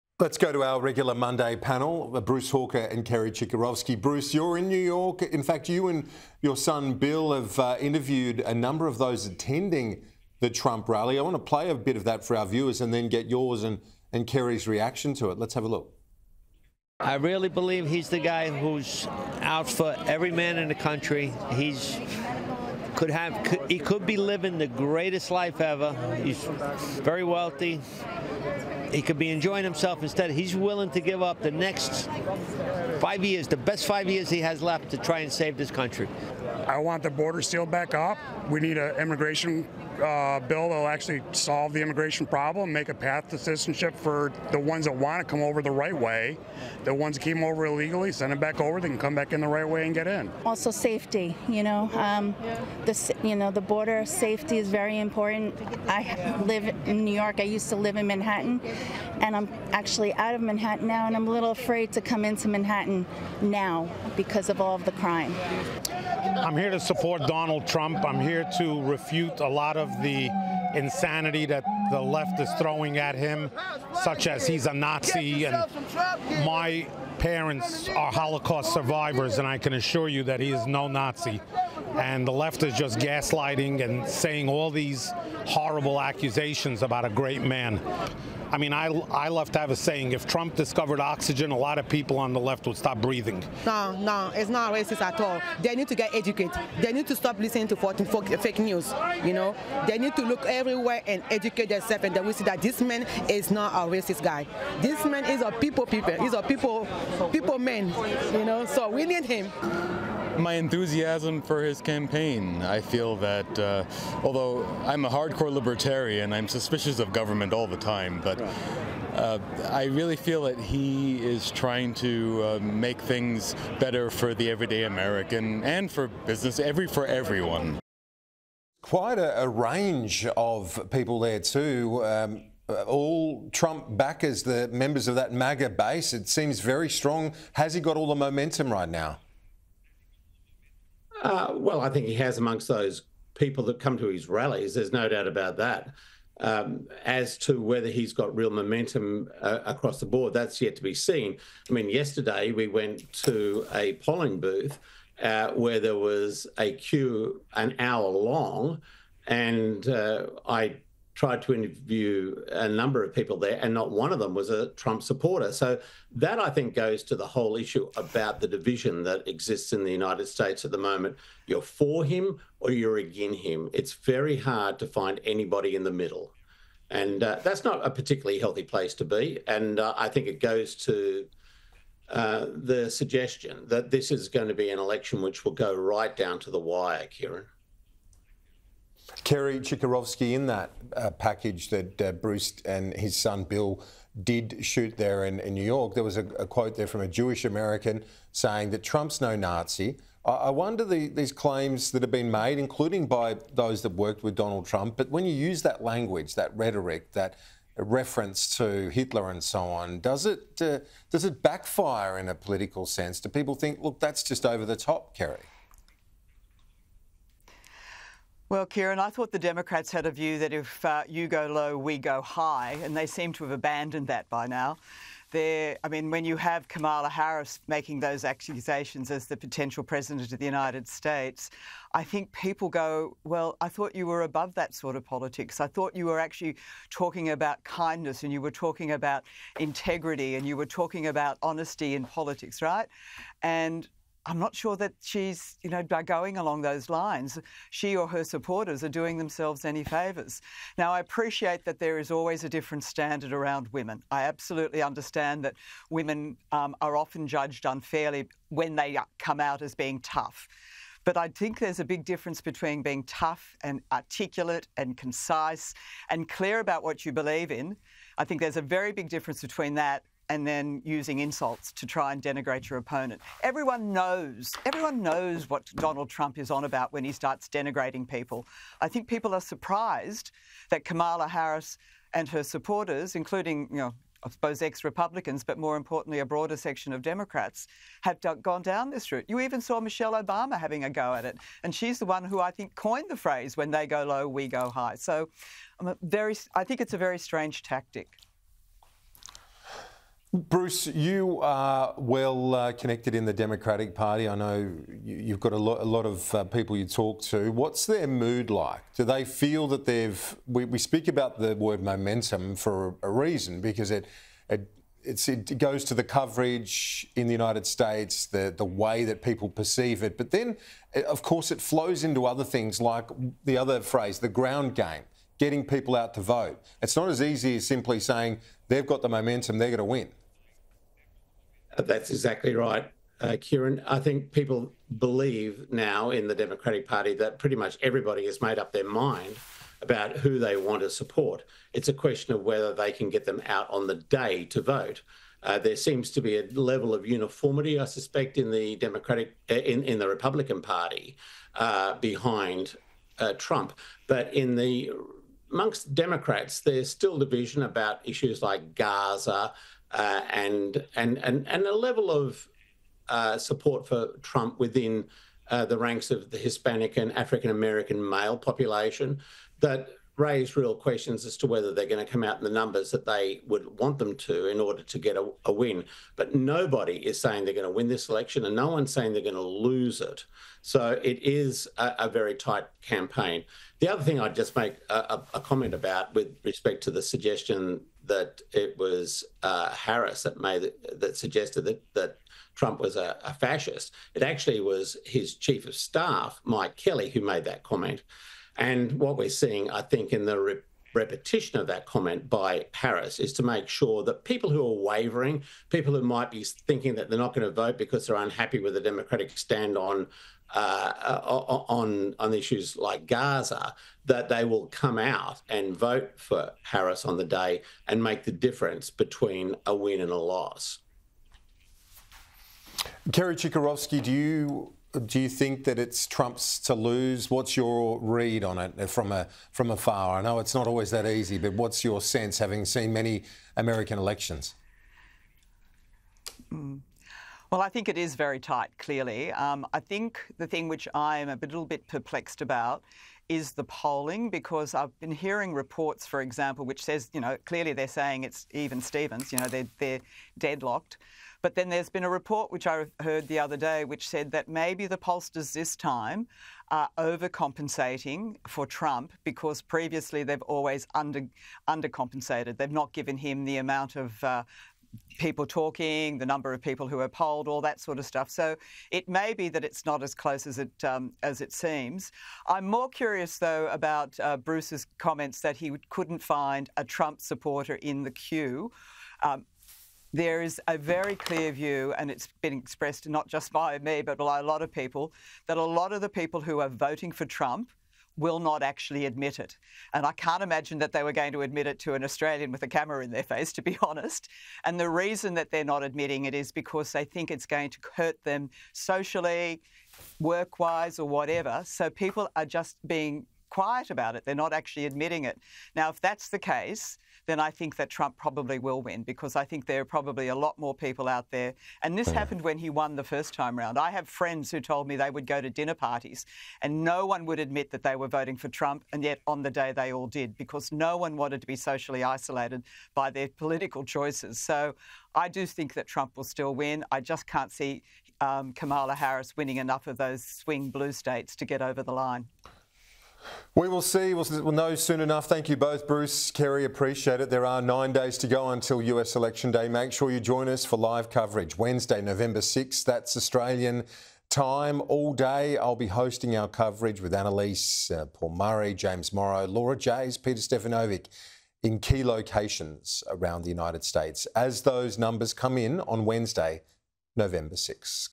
Kamala Harris using insults to try to ‘denigrate’ Donald Trump
Former New South Wales Liberal leader Kerry Chikarovski has slammed US Vice President Kamala Harris for using insults to try and “denigrate” Donald Trump.

Republican presidential candidate Donald Trump held a rally at Madison Square Garden in New York.

“Everyone knows what Donald Trump is on about when he starts denigrating people,” Ms Chikarovski told Sky News Australia.

“I think people are surprised that Kamala Harris and her supporters … have gone down this route.”